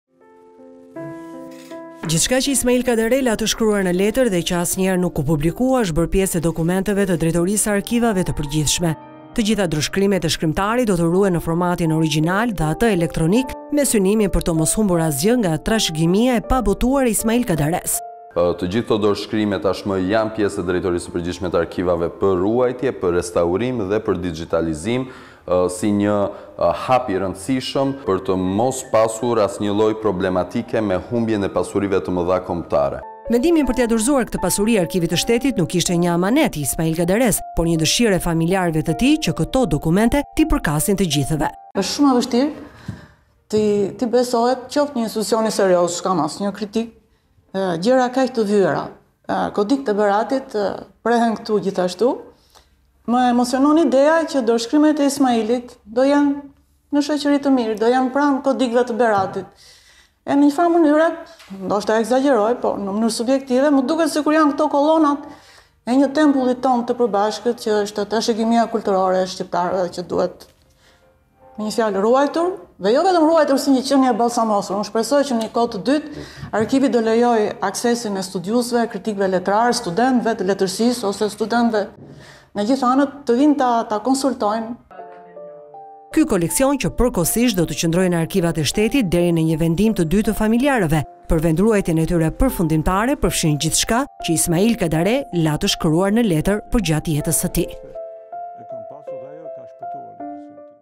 Dhurohen dorëshkrimet e Kadaresë, familjarët i kalojnë Arkivës Shtetërore trashëgiminë e pabotuar
Gjithçka që Ismail Kadare la të shkruar në letër dhe që asnjëherë nuk u publikua është bërë pjesë e dokumenteve të Drejtorisë së Arkivave të Përgjithshme. Të gjitha dorëshkrimet e shkrimtarit do të ruhen në formatin origjinal dhe atë elektronik me synim për të mos humbur asgjë nga trashëgimia e pabotuar e Ismail Kadaresë.

“Të gjitha këto dorëshkrime janë pjesë e arkivave për ruajtje, restuarim dhe digjitalizim si një hap i rëndësishëm për të mos pasuë asnjë lloj problematike me humbjen e pasuirve të mëdha kombëtare”, tha Ardit Bido, drejtor i Arkivave.

Vendimi për t’ia dorëzuar këtë pasuri arkivit të shtetit nuk ishte një amanet i Ismail Kadaresë por një dëshirë e familjarëve të tij, që këto dokumente t’i përkasin të gjithëve.

Është shumë e vështirë t’i besohet një institucioni gjëra kaq të vyera. Këtu prehen kodikët e Beratit, më emocionin ideja që dorëshkrimet e Ismailit do të jenë në shoqëri të mirë, do të jenë pranë kodikëve te Beratit. Është trashëgimia kulturore e shqiptarëve që duhet ruajtur dhe jo vetm duhet ruajtur si një qënie e ballsamosur, unë shpresoj që në një kohë të dytë arkivi do të lejojë aksesin e studiuesve, kritikëve letra dhe studentëve që t’i konsultojnë dhe t’i studiojnë, u shpreh e bija e shkrimtarit, Gresa kadare.

Ky koleksion që përkohësisht do të qëndrojë në arkivat e shtetit deri në një vendim të dytë të familjarëve për vendruajtjen e tyre përfundimtare përfshin gjithçka që Ismail Kadare la të shkruar në letër përgjatë jetës së tij.